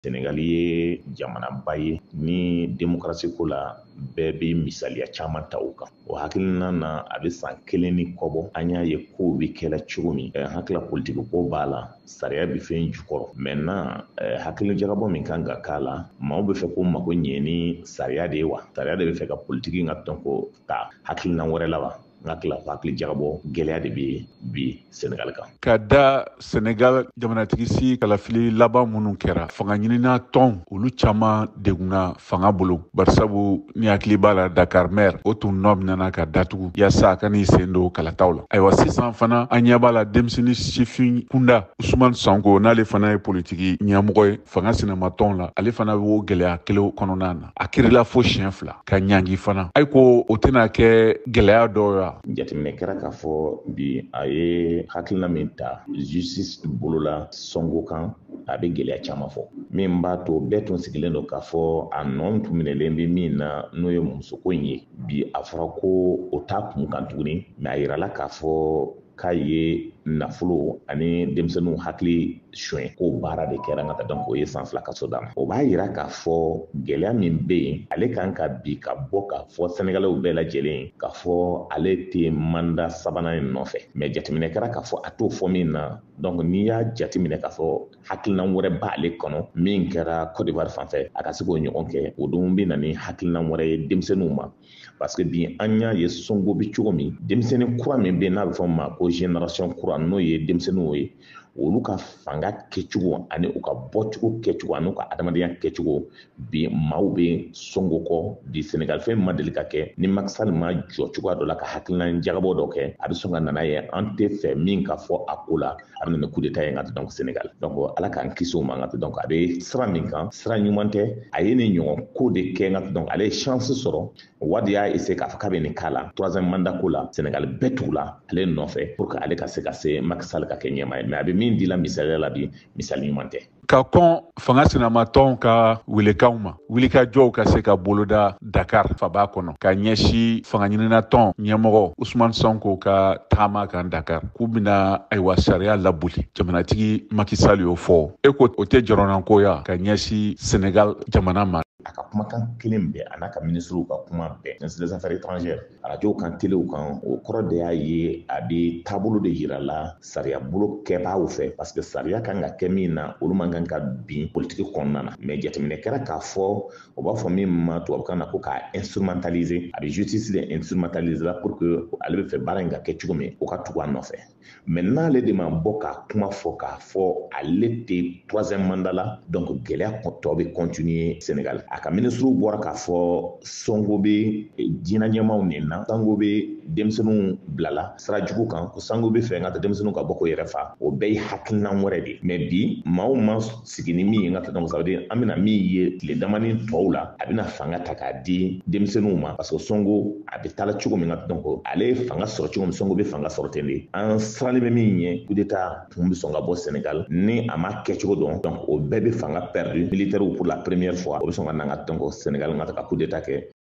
USR politiki ton nyemogo, Nouhoum Togo be hakilijakabo ke Senegal politiki geleya kan
USR politiki ton nyemogo, Nouhoum Togo ka bo, Bamako be hakilijakabo ani tomoni ke, Senegal politiki geleya lahaalayaw kan.